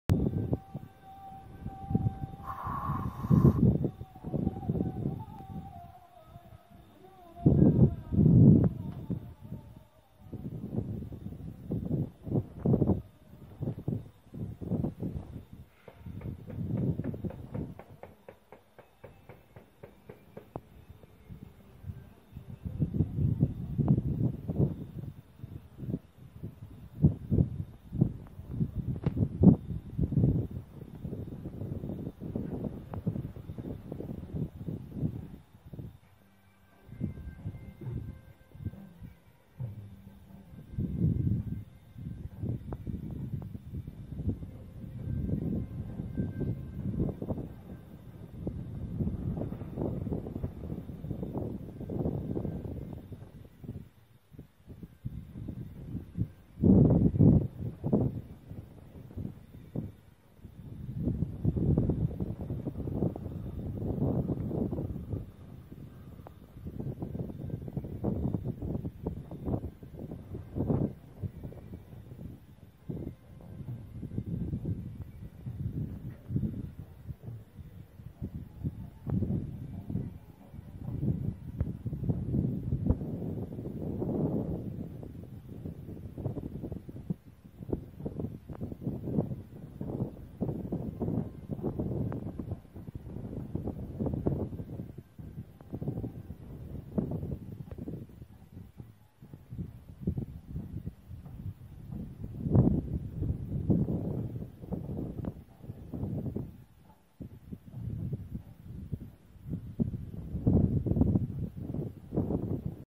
Scorpion sting itself
Akrebin kendini sokmasıyla ilgili basit bir deney yaptım. Farklı görsel ve yazılı kaynaklarda ateş, yüksek sıcaklık veya alkole karşı böyle bir tepki verdiklerinden bahsedilmiş. Deney açık ve serin bir havada aktif maddesi Prallethrin ve d-Tetramethrin olan bir spreyle yapıldı. Akreplerin kimyasal olarak kendi zehrine karşı bağışıklığının olduğuda biliniyor. Kimyasal olarak kendini zehirlemese bile iğnesini kendi bedenine batırdığı görülüyor.